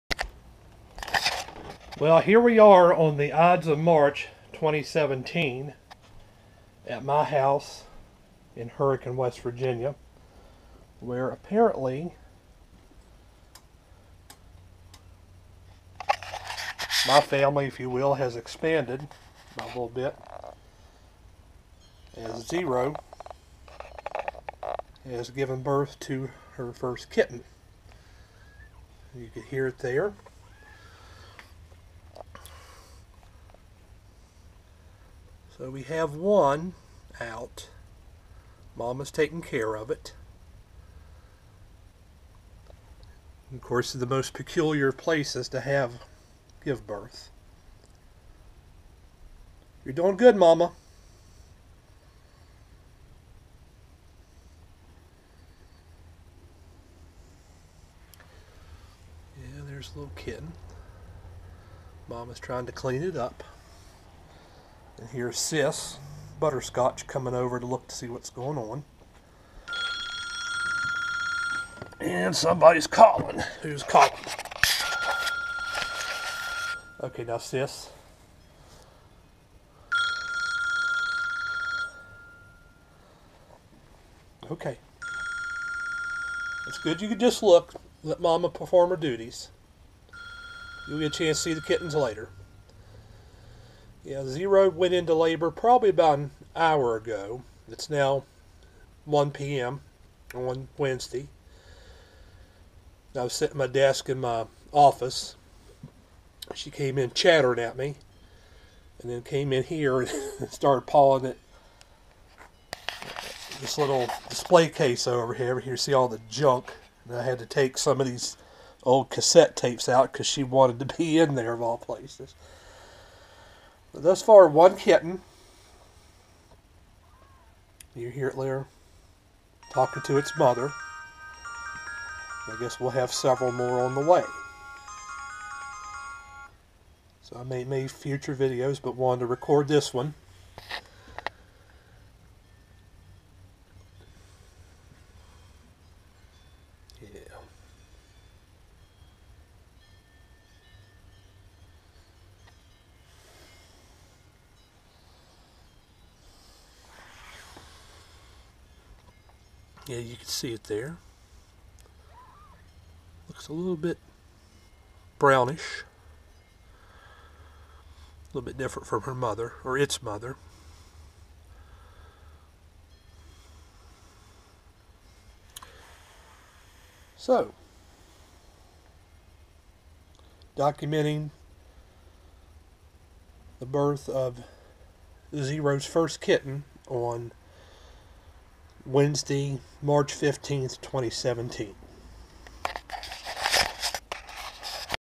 Zero gives birth to kittens - 03/15/2017
Footage of my youngest female cat, Zero, giving birth to a litter of kittens on Wednesday, March 15, 2017